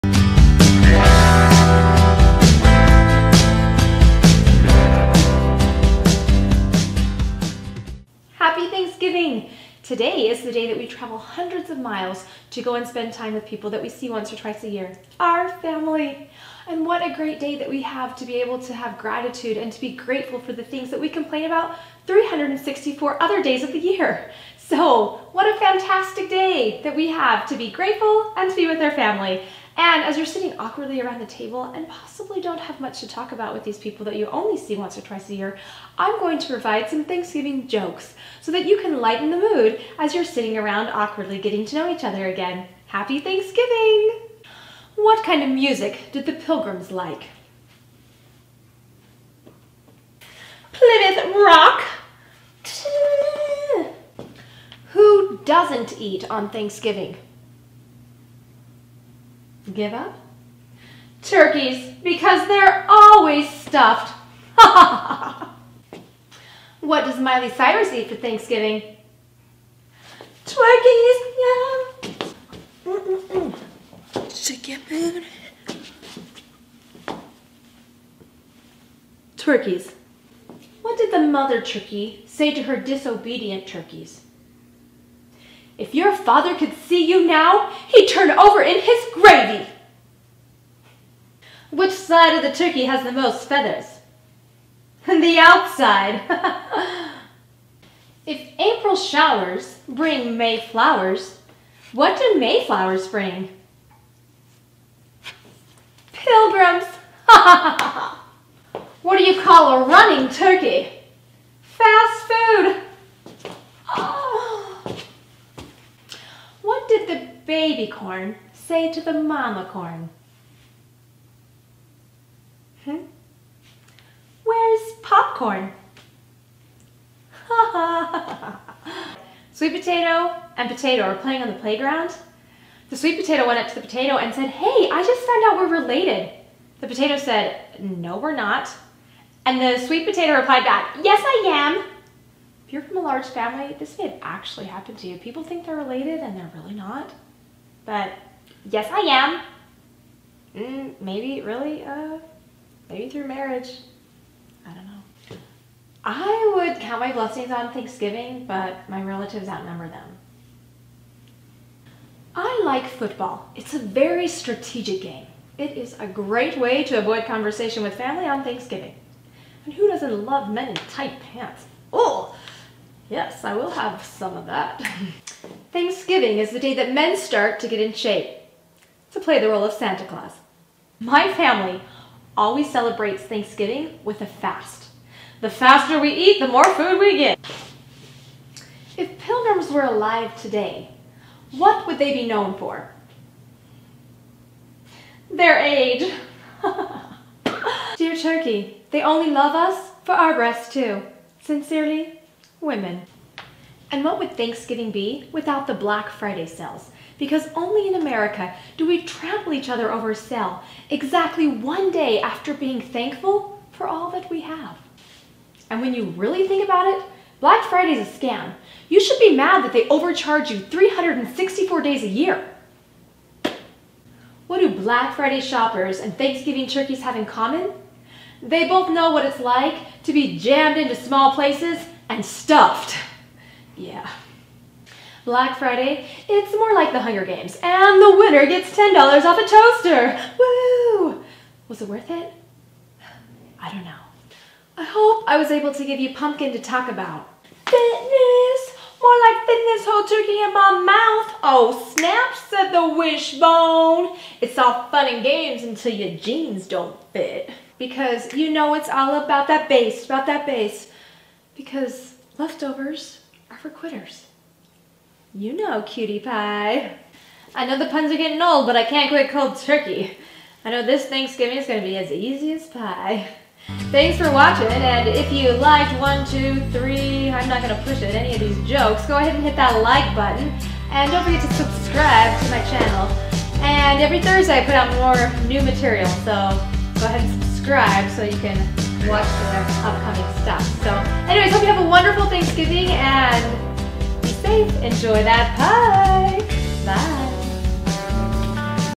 Thanksgiving jokes 2017
How to get through Thanksgiving. Amannda shares some Thanksgiving jokes to help you get through that uncomfortable Thanksgiving dinner with the family.